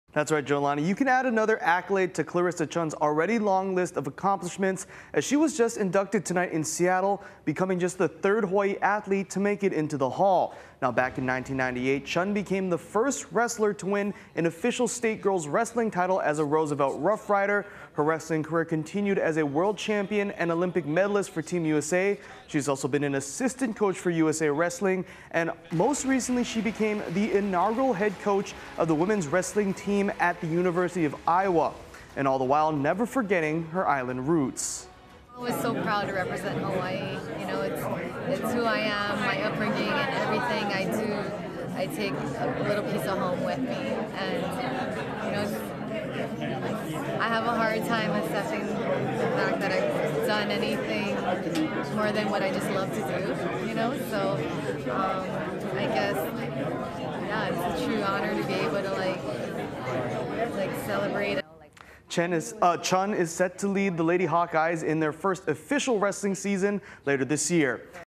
Hawaii’s Clarissa Chun inducted into National High School Sports Hall of Fame
You can add another accolade to Clarissa Chun’s already long long list of accomplishments